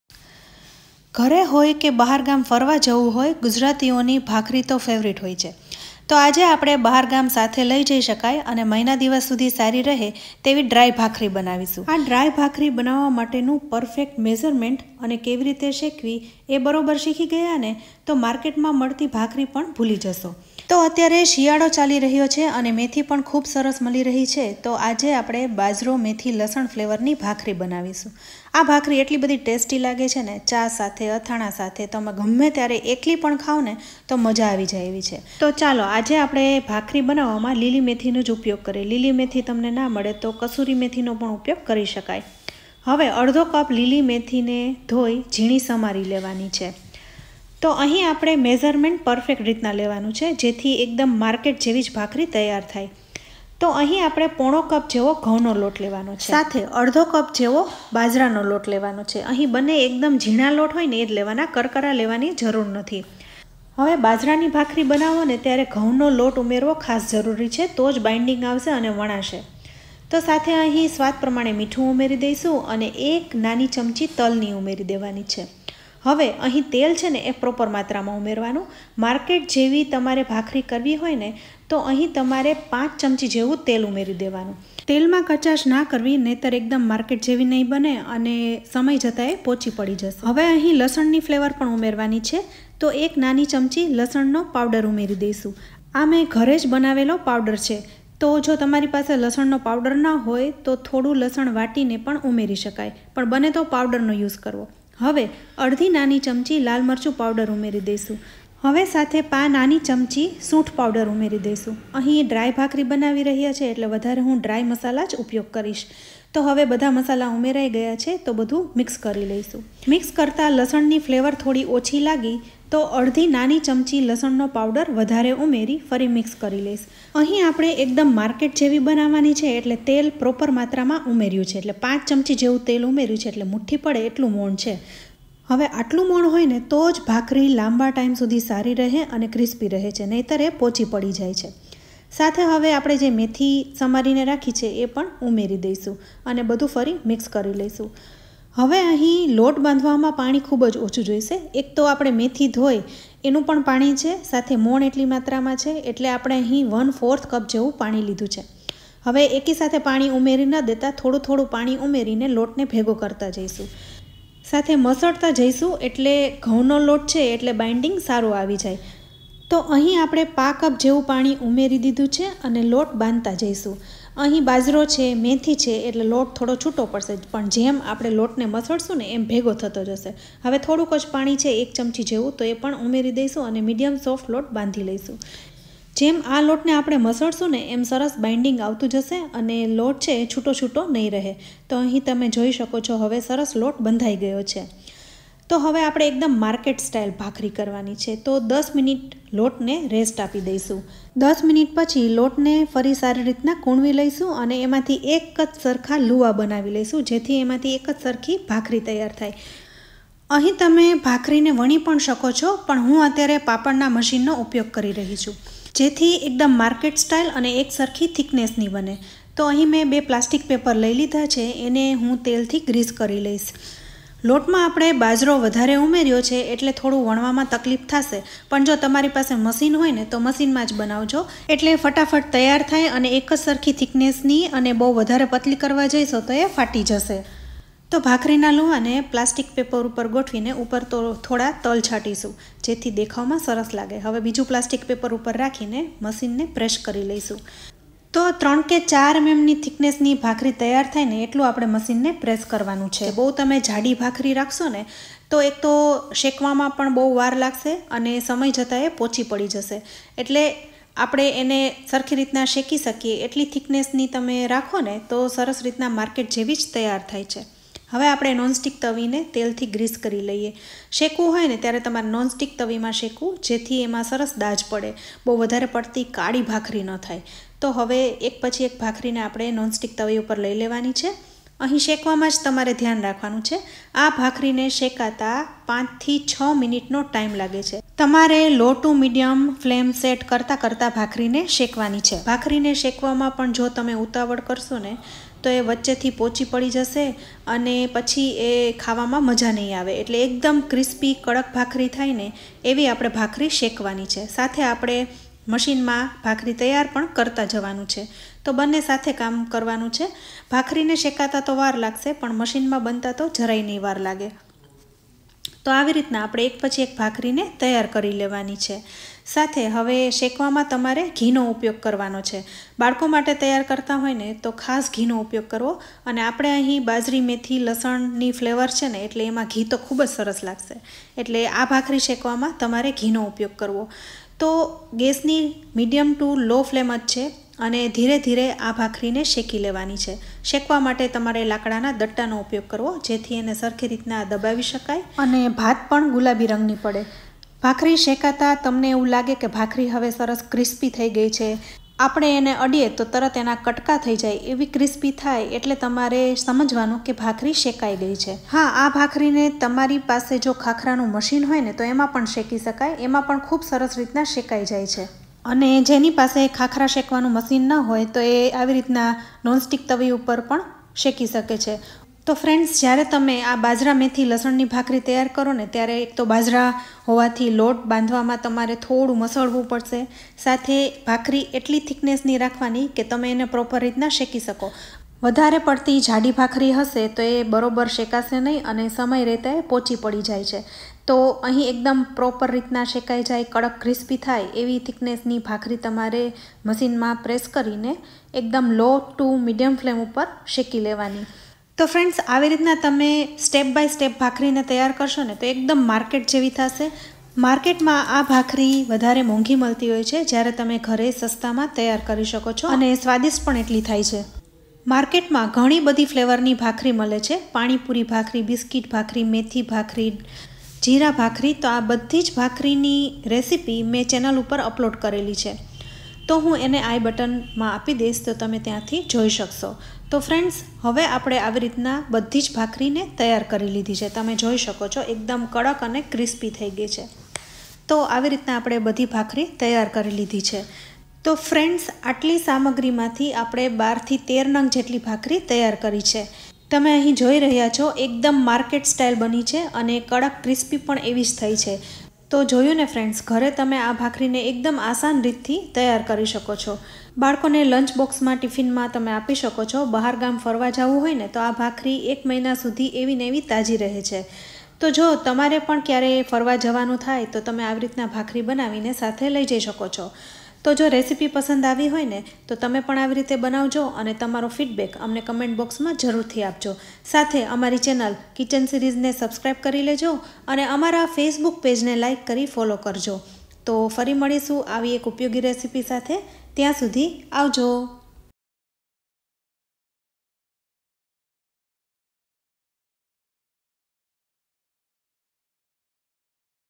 એકદમ માર્કેટ જેવી નવી જ ફ્લેવર ની ડ્રાય ભાખરી જે મહિના સુધી સારી રહે /methi bajri lasan dry bhakhri
Hello friends
today i am sharing market style crispy dry bhakhri khakhra recipe which flavour is very tasty here i have made bajri methi garlic bhakhri …. bhakhri gujrati’s all time favourite food so this dry bhakhri you can store for whole month and remains crispy and freash so friends try out this dry bhakhri recipe here are all ingredients:

1/2 cup green methi
3/4 cup wheat flour
1/2 cup bajri flour
5 tbsp oil
salt to taste
1 +1/2 tsp garlic powder
1/2 tsp red chilly powder
1 tsp seasome seeds
1/4 cup water
ghee/oil to roast

Please do try this recipe and convey your valuable feedback in the comment section below. Thanking you. Have a great day.